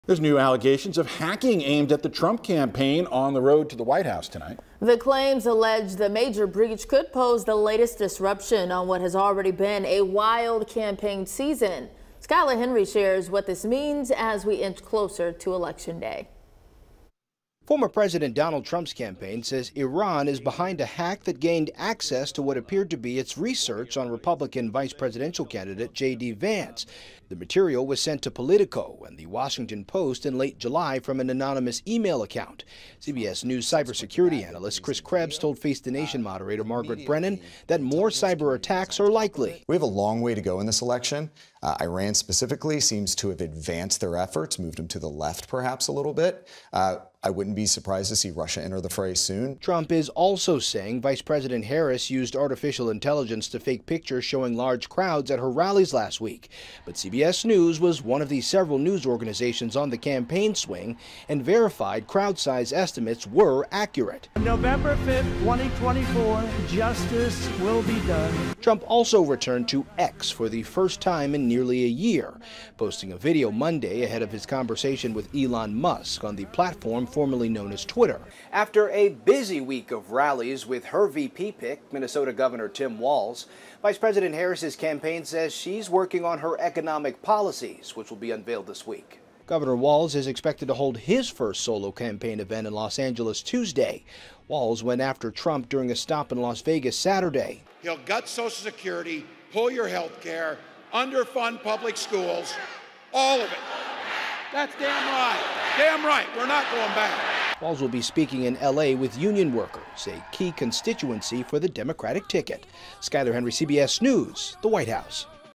New allegations of hacking aimed at the Trump campaign | What we know
New allegations have emerged of a potential hacking aimed at the Trump campaign on the road to the White House.